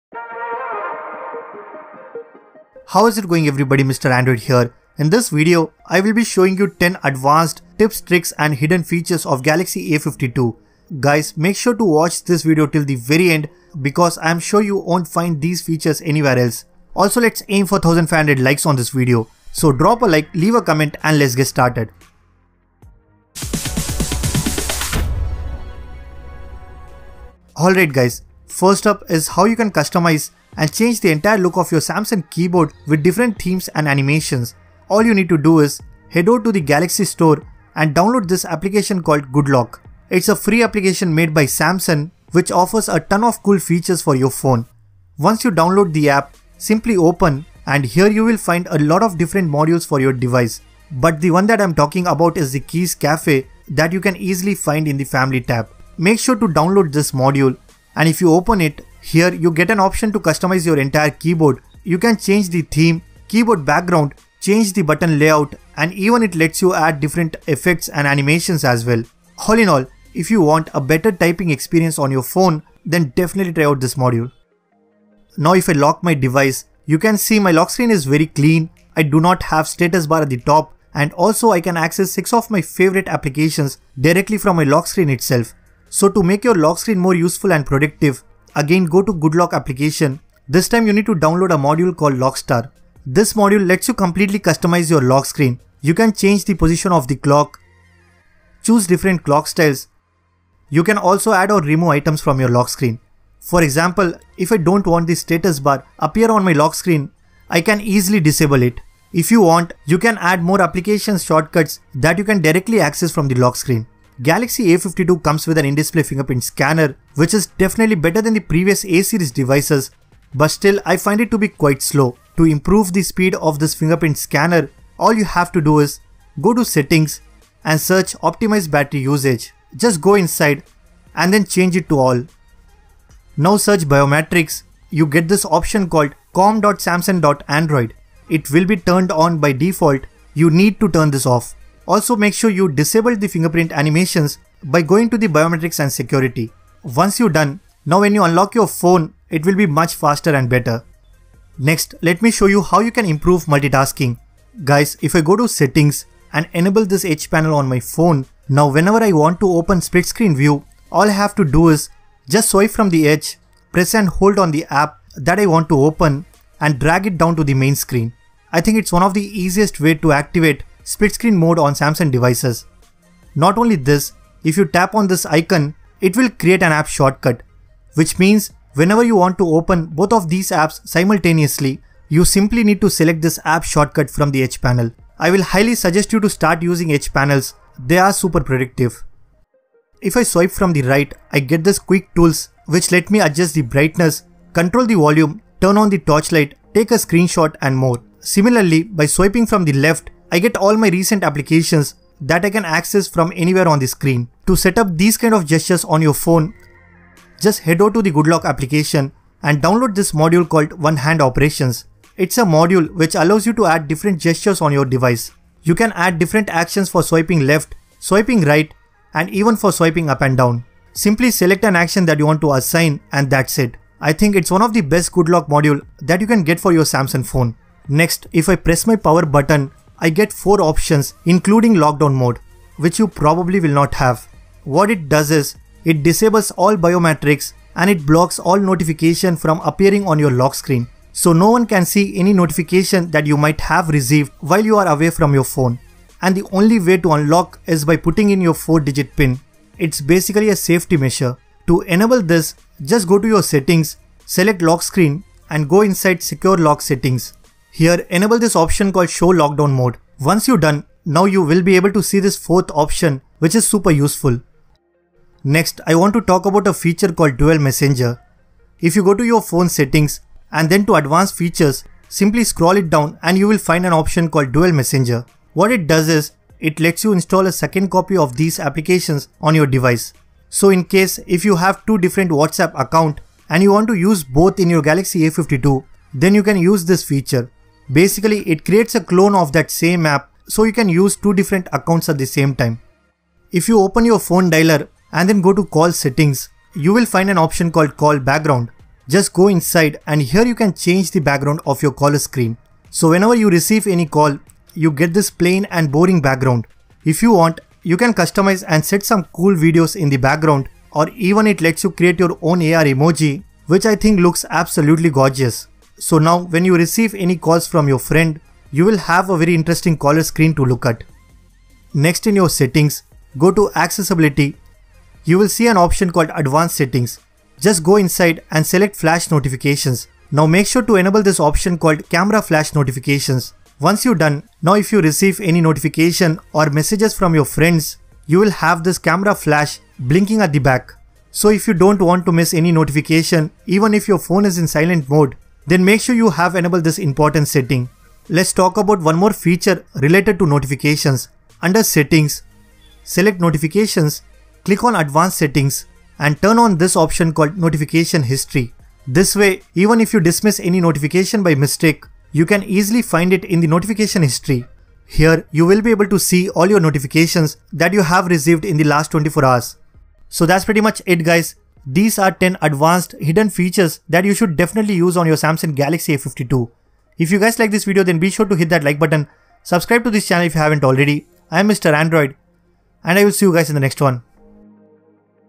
Samsung Galaxy A52 - 10 Advanced Hidden Features You should Know!
In this video, I will be showing you 10 Advanced Hidden Features of Samsung Galaxy A52. All these are very useful Features that you should know, if you own Galaxy A52.

Please do join our Telegram Group for some exclusive wallpapers and tech news: